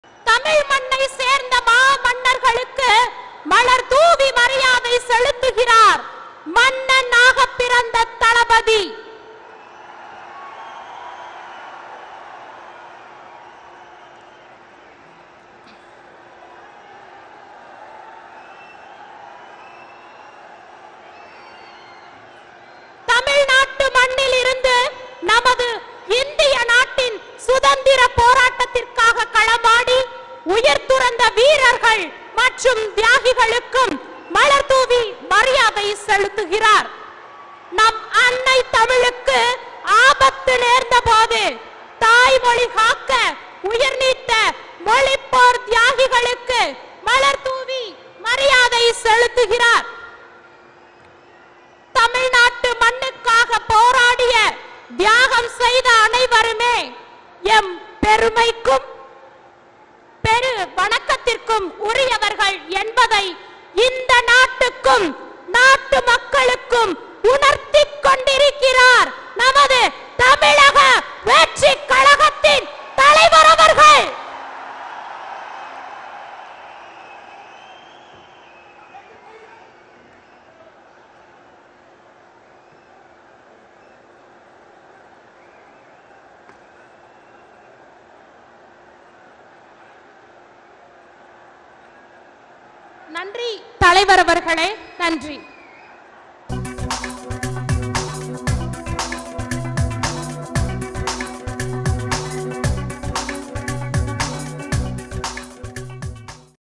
மொழிப்போர் தியாகிகளுக்கு மலர் தூவி அஞ்சலி செலுத்திய விஜய்.. சல்யூட் மொமெண்ட்..!!
சுதந்திர போராட்ட தியாகிகள் மற்றும் மொழிப்போர் தியாகிகளுக்கு மலர் தூவி அஞ்சலி செலுத்திய விஜய்.. சல்யூட் மொமெண்ட்..!!